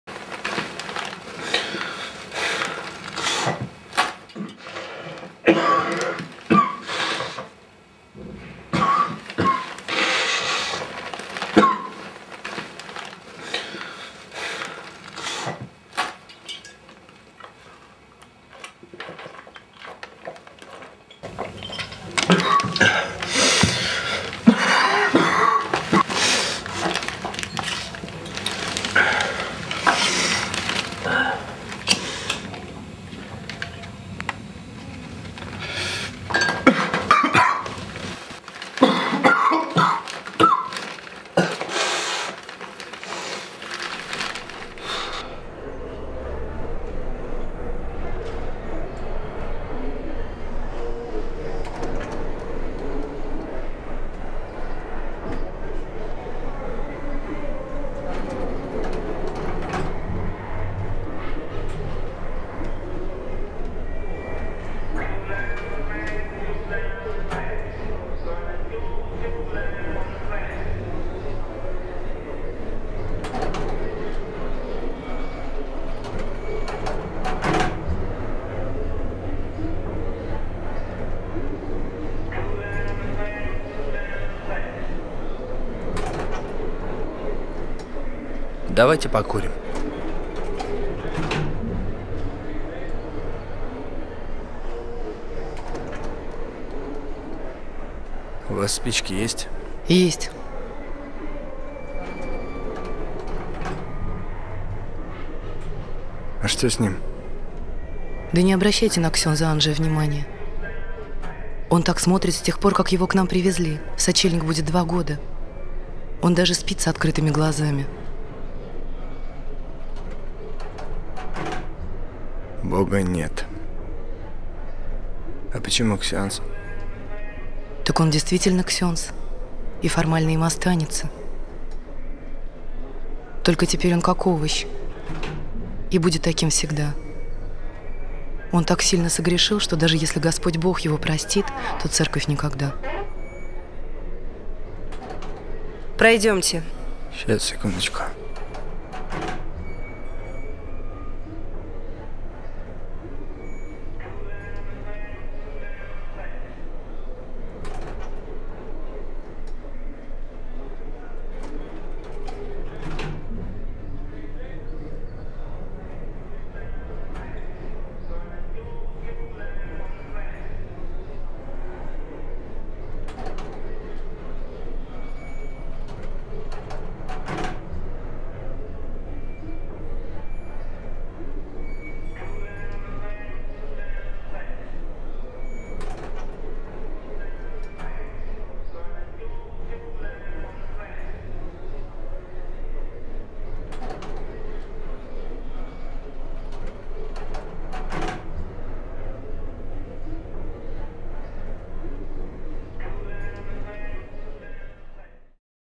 Scene at mental institution from a play "Loneliness in the net" directed by Henryk Baranowski
Scene at mental institution from a play "Loneliness in the net" written by J. L. Wiśniewski and directed by Henryk baranowski. Camera and editor Svetlana Bakushina. Baltic-house theatre, St Petersburg, 2009. This video is used as video stage projection. Thank you!